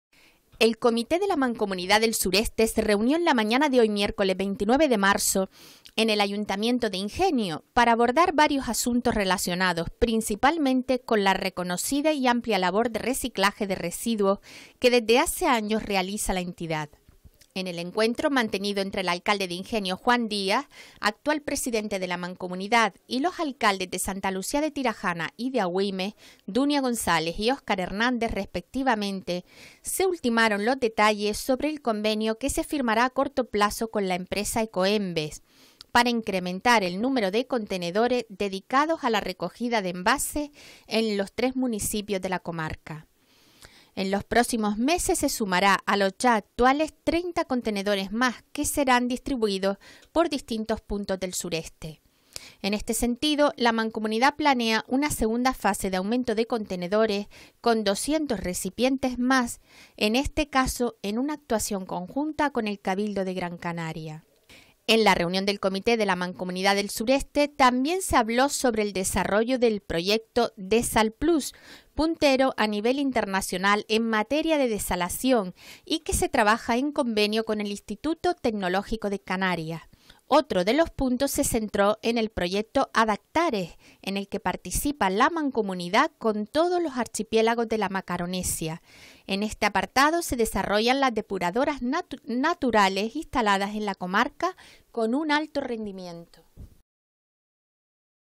La Mancomunidad del Sureste sumará este año 200 nuevos contenedores de reciclaje
El Comité de la Mancomunidad del Sureste se reunió en la mañana de hoy miércoles, 29 de marzo, en el Ayuntamiento de Ingenio para abordar varios asuntos relacionados, principalmente, con la reconocida y amplia labor de reciclaje de residuos sólidos que desde hace años realiza la entidad. En el encuentro mantenido entre el alcalde de Ingenio, Juan Díaz, actual presidente de la Mancomunidad, y los alcaldes de Santa Lucía de Tirajana y de Agüimes, Dunia González y Óscar Hernández, respectivamente, se ultimaron los detalles sobre el convenio que se firmará a corto plazo con la empresa Ecoembes, para incrementar el número de contenedores dedicados a la recogida de envases en los tres municipios del sureste. Por lo que en los próximos meses se sumará a los ya actuales, 30 contenedores más que serán distribuidos por distintos punto del sureste.  En este sentido, la Mancomunidad planea una segunda fase de aumento de contenedores con 200 recipientes más, en este caso en una actuación conjunta con el Cabildo de Gran Canaria.

En la reunión del Comité de la Mancomunidad del Sureste también se habló sobre el desarrollo del proyecto Desal +, puntero a nivel internacional en materia de desalación y que se trabaja en convenio con el Instituto Tecnológico de Canarias. Otro de los puntos se centró en  el proyecto 'Adaptares', en el que participa la Mancomunidad del Sureste con todos los archipiélagos de La Macaronesia. En este apartado se desarrollan las depuradoras naturales instaladas en la comarca con un alto rendimiento.